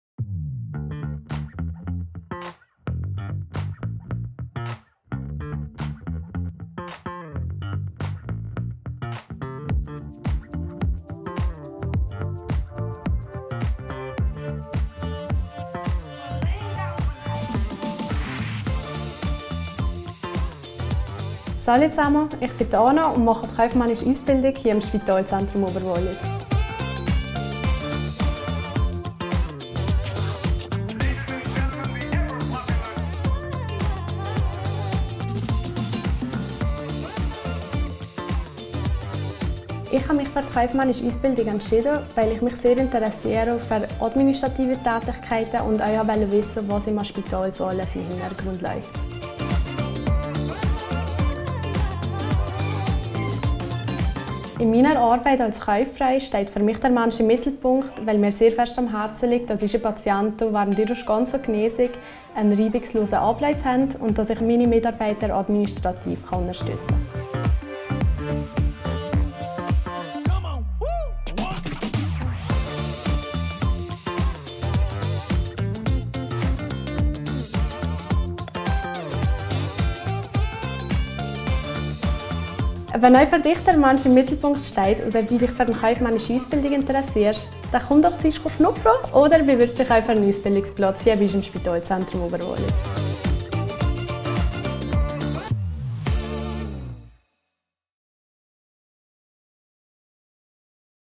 Lehre als Kaufmann/-frau im Spitalzentrum Oberwallis
Ein Einblick in den Alltag einer Lehrstelle im Spitalzentrum Oberwallis in Brig/Visp, (VS) als Kaufmann/-frau EFZ